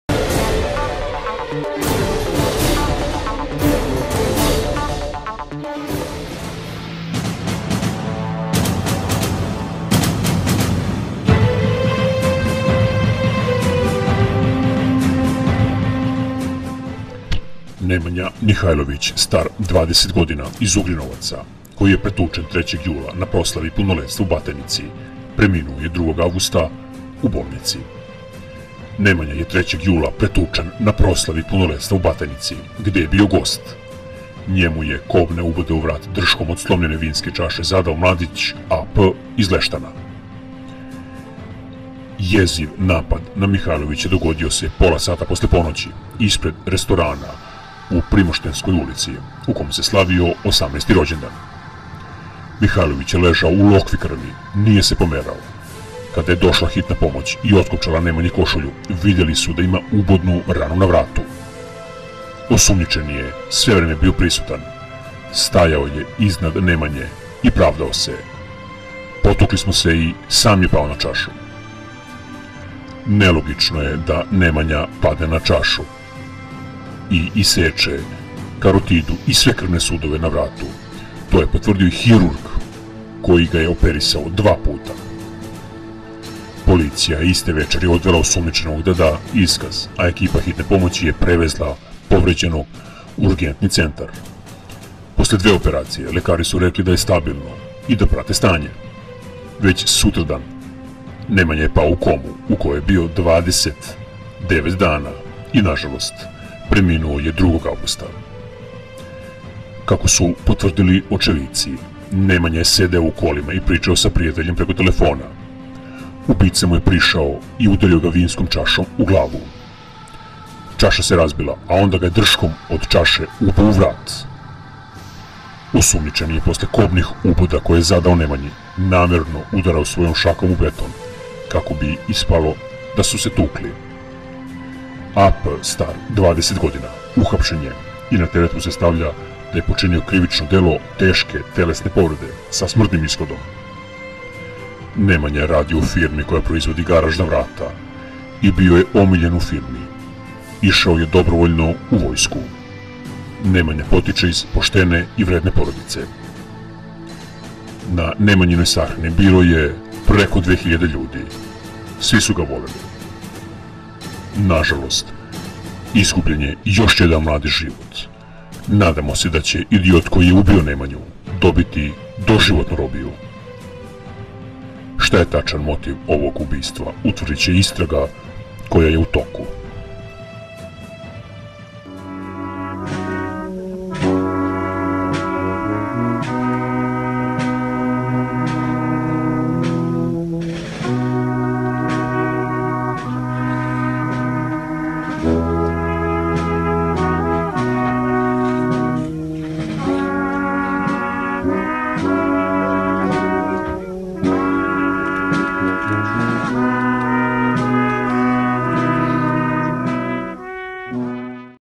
NEMANJA MIHAJLOVIĆ(20) 02.08.2023
Nemanja Mihajlović (20) iz Ugrinovaca, koji je pretučen 3. jula na proslai punoletstva u Batajnici, preminio je 2 avgusta u bolnici.
Nemanja je 3. jula pretučen na proslavi punoletstva u Batajnici, gde je bio gost!  njemu je kobne ubode u vrat drškom od slomljene vinske čaše zadao mladić A. P. iz Leštana.

Jeziv napad na Mihajlovića dogodio se pola sata posle ponoću ispred restorana u Primoštenskoj ulici, u kom se slavio 18. rođendan.
Mihajlovic je lezao u lokvi krvi,nije se pomerao,kada je došla Hitna i otkopčala Nemanji košulju, videli su da ima ubodnu ranu na vratu.
osumnjičeni je sve vreme bio prisutan, stajao je iznad  Nemanje i pravdao se: "Potukli smo se i sam je pao na čašu".
Nelogicno je da Nemanja  padne na čašu i iseče karotidu i sve krvne sudove na vratu do kičmene moždine ,to je potvrdio i hirurg koji ga je operisao dva puta.
policija je iste večeri odvela osumnjičenog da da iskaz, a ekipa Hitne pomoći je prevezla povređenog u Urgentni centar.
Posle dve operacije, lekari su rekli da je stabilno i da pratimo stanje. Već sutradan je pao u komu u kojoj je bio 29 dana,i nazalost Nemanja je preminuo 2 avgusta.
Kako su potvrdili ocevici.Nemanja je sedeo u kolima i pričao sa prijateljem preko telefona. Ubica mu je,  prišao i udario vinskom čašom u glavu. Čaša se razbila, a onda ga je drškom od čaše ubo u vrat.
osumnjičeni, je posle kobnih uboda koje je zadao Nemanji , namerno udarao svojom šakom u beton kako bi ispalo da su se tukli.
A. P.(20) je uhapšen i na teret mu se stavlja da je počinio krivično delo teške telesne povrede sa smrtnim ishodom
Nemanja je radio u firmi koja proizvodi garažna vrata i da je bio omiljen u firmi,isao je io dobrovoljno u vojsku,Nemanja potice iz postene i vredne porodice.
Na Nemanjinoj sahrani bilo je 2.000 ljudi, svi su ga voleli.
Nazalost izgubljen je jos jedan mladi zivot,Nadamo se da ce idiot koji je ubio Nemanju dobiti dozivotnu robiju.
Sta je tacan motiv ovog ubistva ,utvrdice istraga koja je u toku.